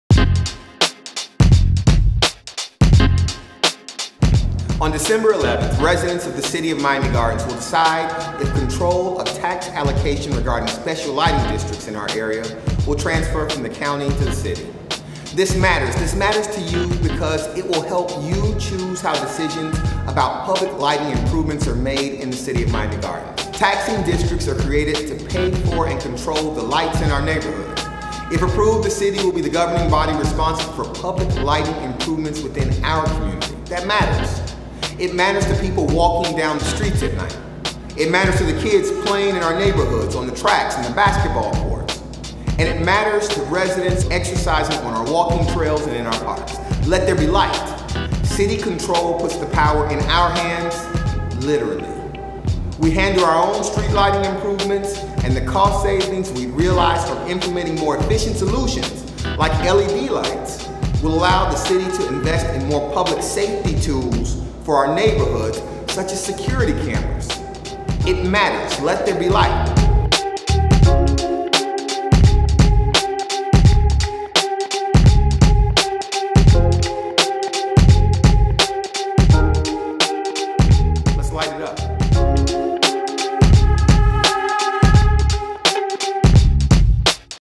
Mayor Oliver Gilbert says "Let There Be Light!"
On December 11, 2018, Miami Gardens Residents
will decide who controls the lights in our City.

Ballots will be mailed on November 21, 2018.

This special election is vote by mail only.